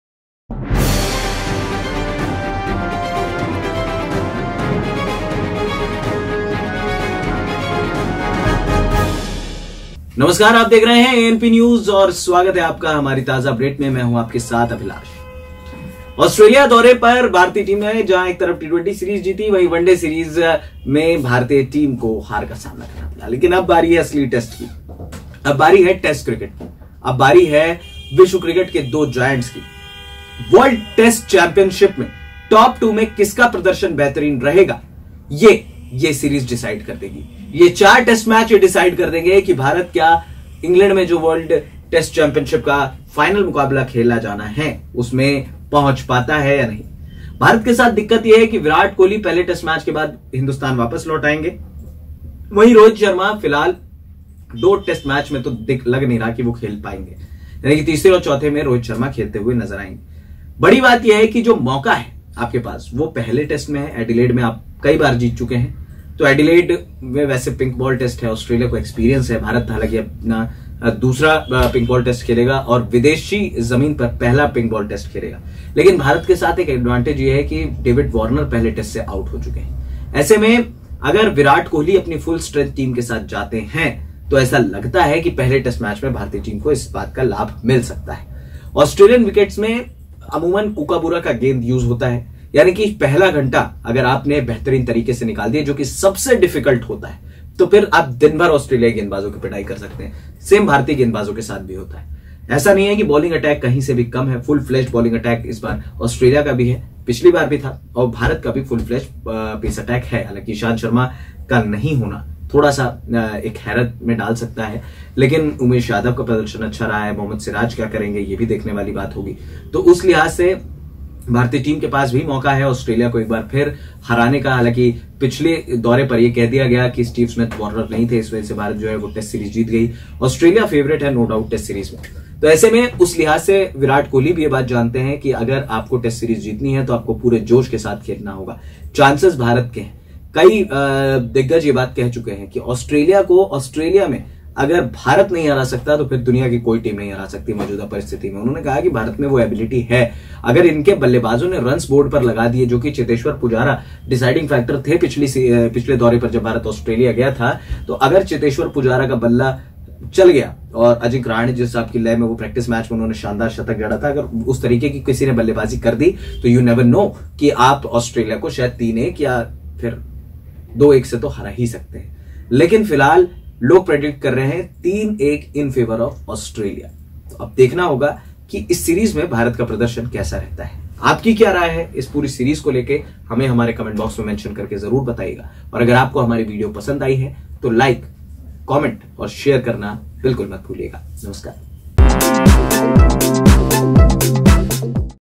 AUSTRALIA के टेस्ट सीरीज से पहले टीम इंडिया को मिली GOOD NEWS!4-0 से टेस्ट सीरीज जीतेगा भारत
इस ख़ास रिपोर्ट में देखें AUSTRALIA के टेस्ट सीरीज से पहले टीम इंडिया को मिली GOOD NEWS!4-0 से टेस्ट सीरीज जीतेगा भारत

ANPNEWS
anpnews